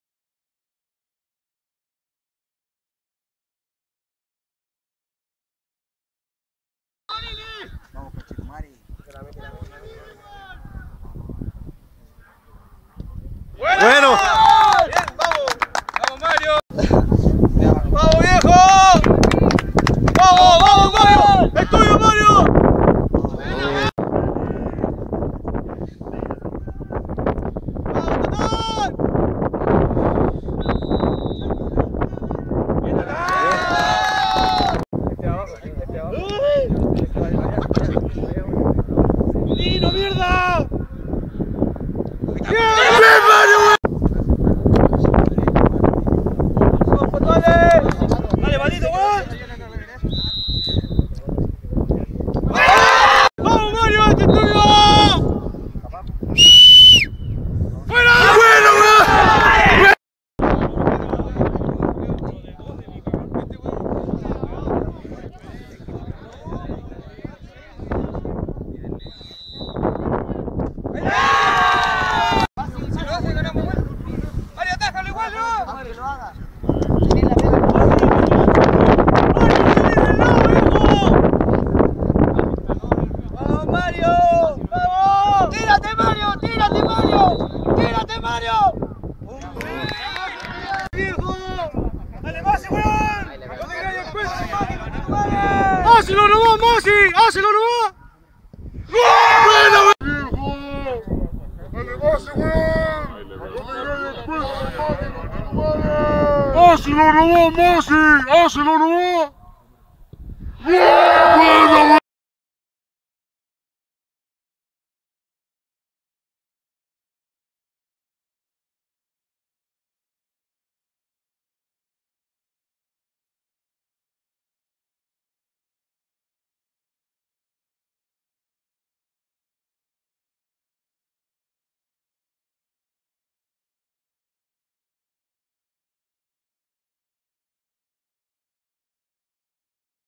Cuartos de Final Torneo Fiestas Patrias 2016
Deportivo Los Mapuche vs Deportivo Micreros (ganador)

Tiempo reglamentario 4x4
Penales: 2x4